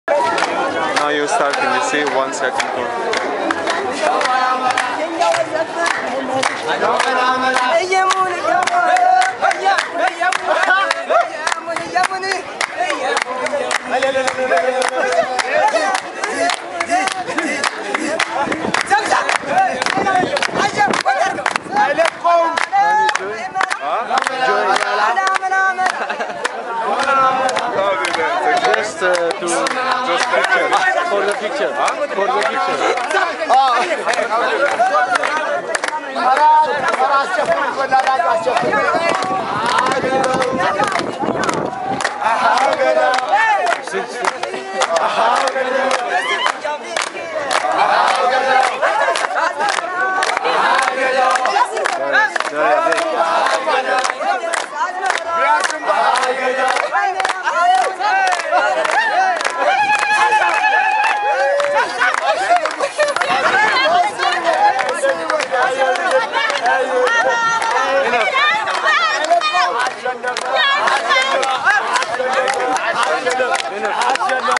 Epiphany Street Dance Addis Ababa 2014
Traditional Epiphany dance recorded during Epiphany celebration in Addis Ababa, Ethiopia